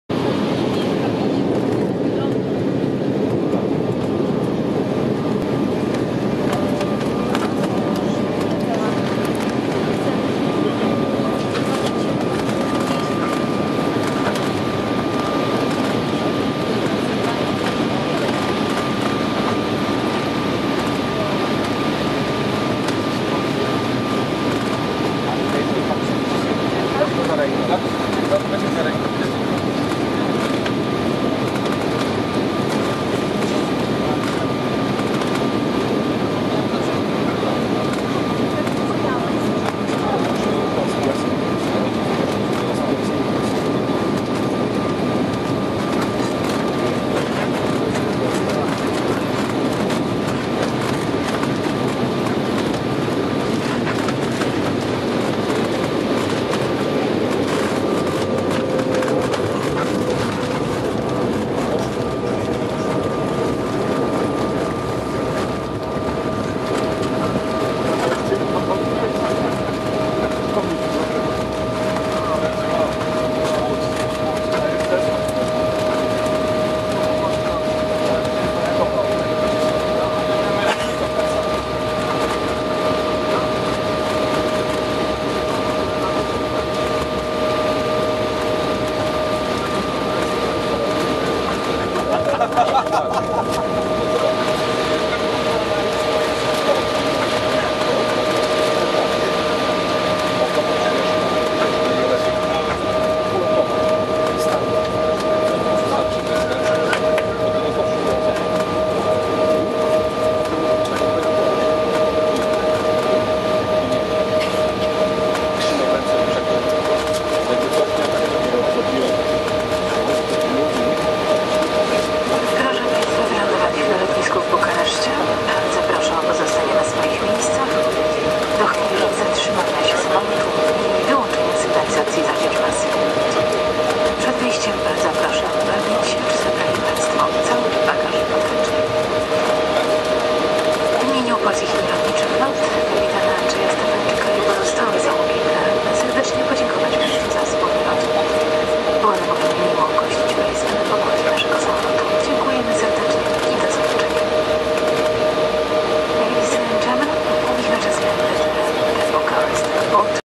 25.01.12 ladowanie w Bukareszcie.MP4
Lądowanie w Bukareszcie, odczuwalne turbulencje